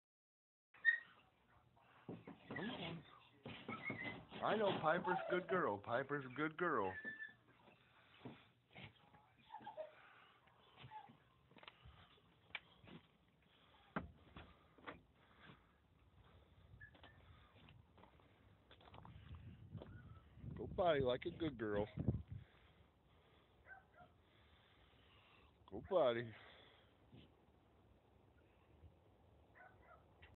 3 legged dog is a champ
Pitbull Oakley is up hopping around right after having to have a leg amputated due to a train accident